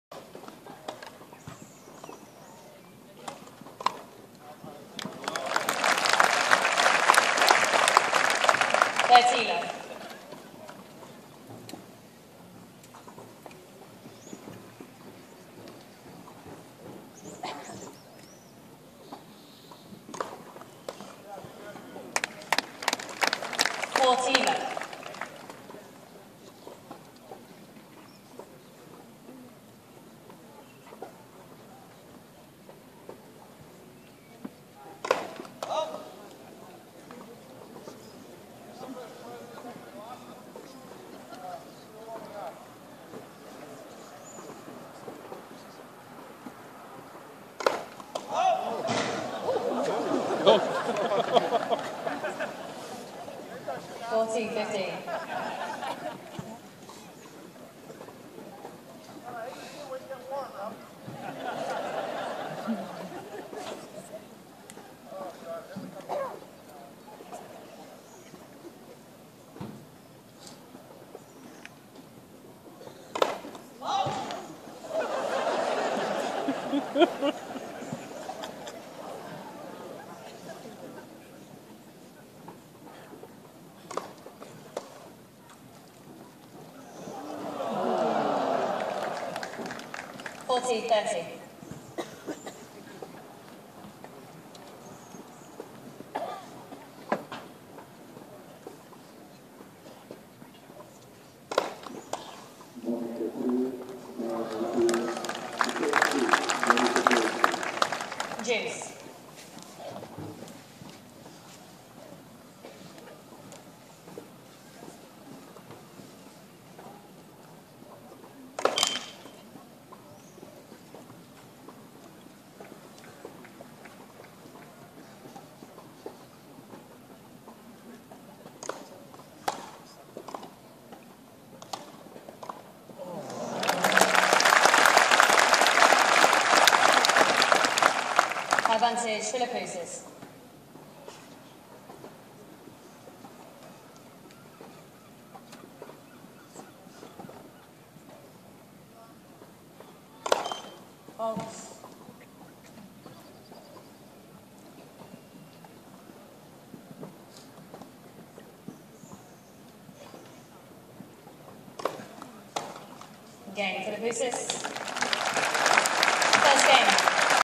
Mark Philippoussis v Greg Rusedski - Exhibition tennis match at Eastbourne 2013
A few points from the Mark Philippoussis v Greg Rusedski - Exhibition match at Eastbourne on June 6th 2013